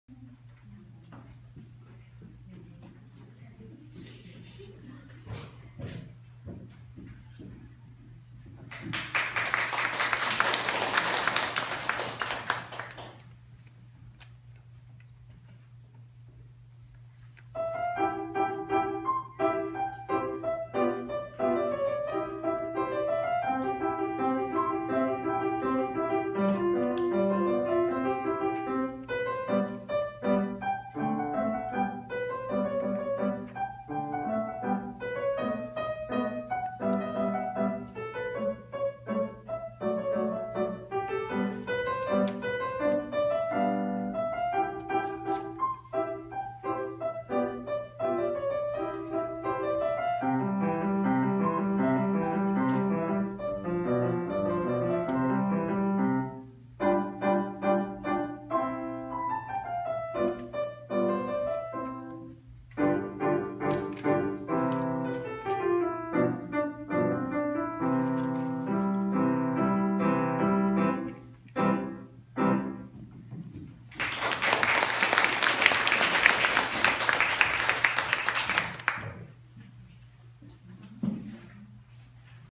Piano competition
Benjamin's first piano competition, 3rd place.